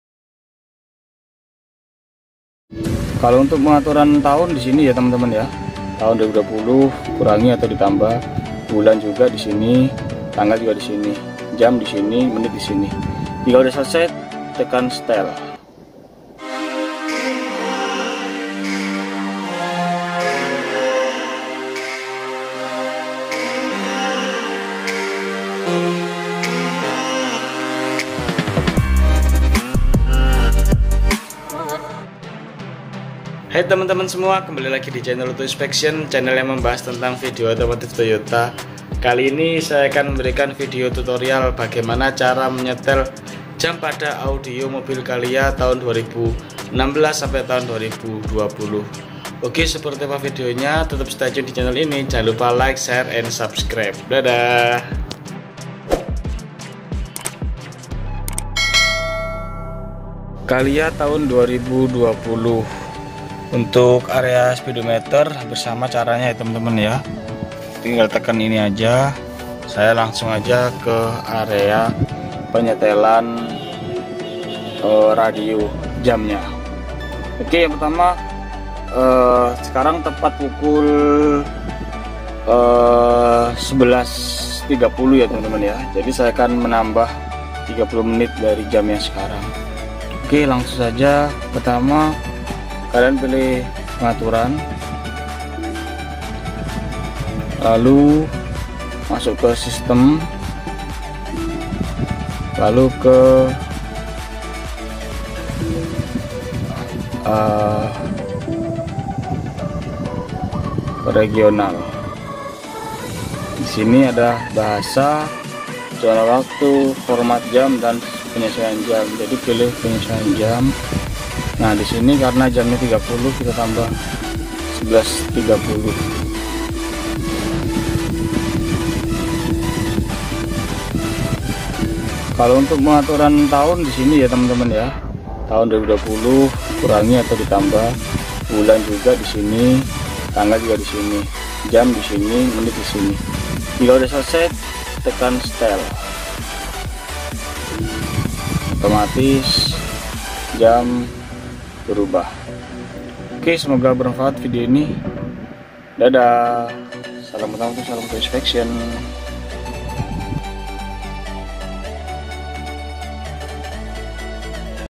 Cara setting jam pada mobil toyota calya 2020
berikut adalah video tutorial bagaimana cara stel jam pada audio calya.

dm for credit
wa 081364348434